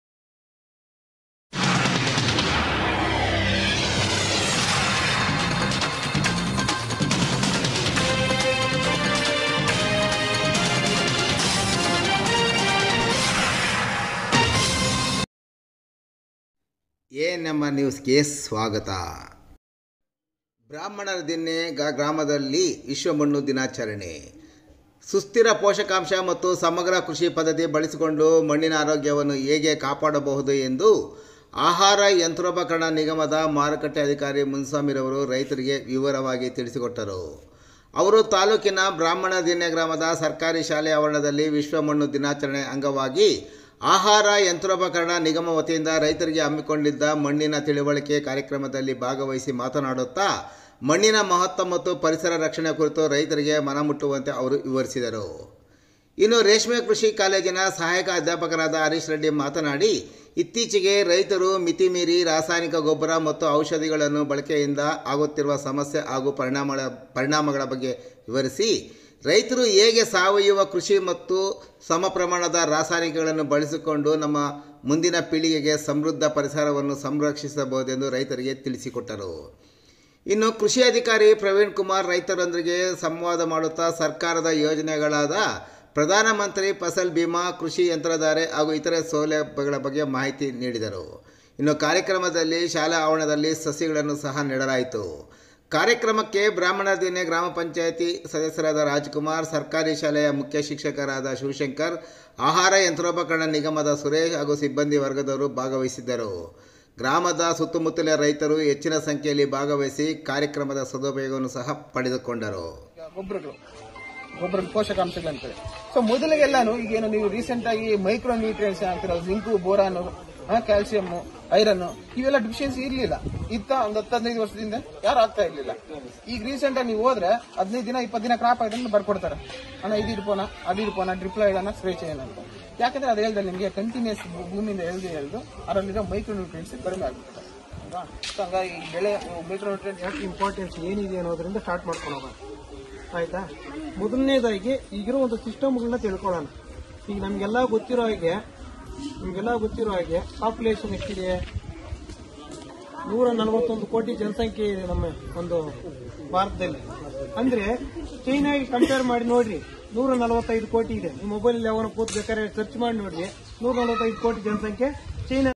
ಚಿಂತಾಮಣಿ ತಾಲ್ಲೂಕಿನ ಬ್ರಾಹ್ಮಣರದಿನ್ನೆ ಗ್ರಾಮದಲ್ಲಿ ಆಹಾರ ಯಂತ್ರೋಪಕರಣ ನಿಗಮದಿಂದ ವಿಶ್ವ ಮಣ್ಣು ದಿನಾಚರಣೆ
ಚಿಂತಾಮಣಿ ಬ್ರಾಹ್ಮಣರದಿನ್ನೆ ಗ್ರಾಮ ದಲ್ಲಿ ಆಹಾರ ಯಂತ್ರೋಪಕರಣ ನಿಗಮದಿಂದ ವಿಶ್ವ ಮಣ್ಣು ದಿನಾಚರಣೆ